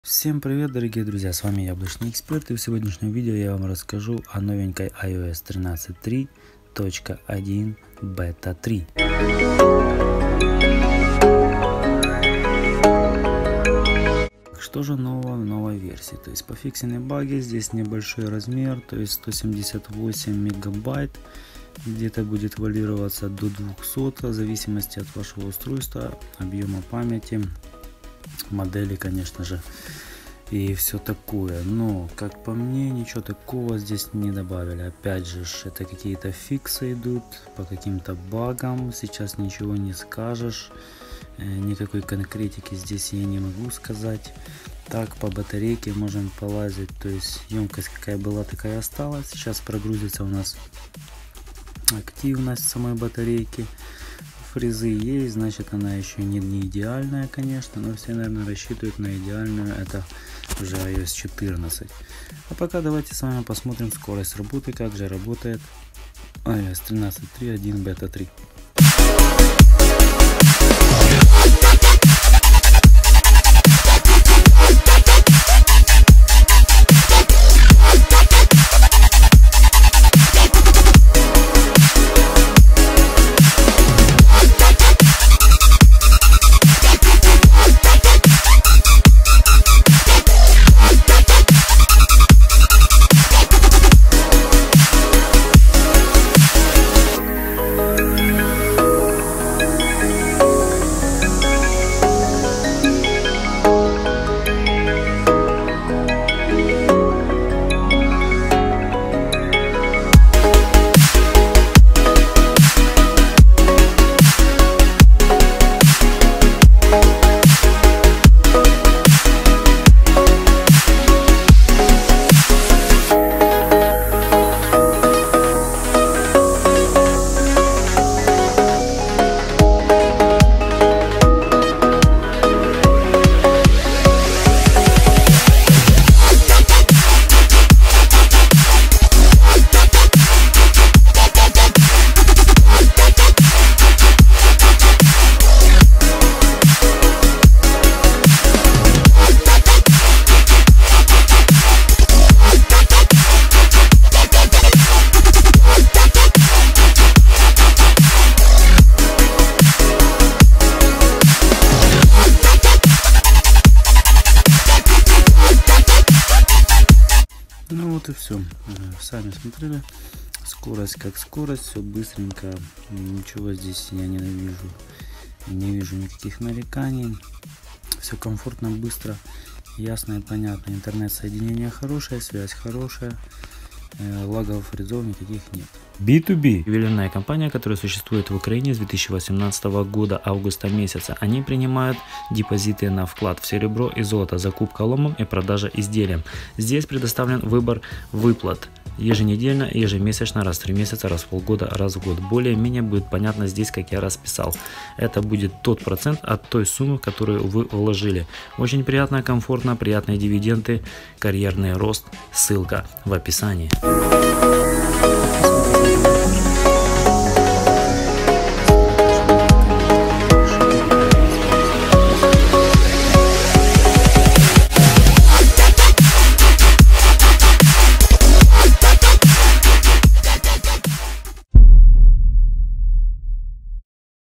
Обзор iOS 13.3.1 бета 3 на iPhone 7 - iApple Expert
Новые фишки iOS 13.3.1 бета 3
Скорость работы iOS 13.3.1 бета 3
Как держит батарейка на iOS 13.3.1 бета 3
Стоит ли устанавливать iOS 13.3.1 бета 3

НЕ ЗАБЫВАЕМ ДЕЛАТЬ РЕЗЕРВНУЮ КОПИЮ В iTunes/iCloud

Главными и новыми фишками стада всего один подпункт это:
1. Исправление багов в системе iOS
2. Повышение автономности
3. Повышение производительности
4. Устранение ошибок с подключением Wi-Fi, Bluetooth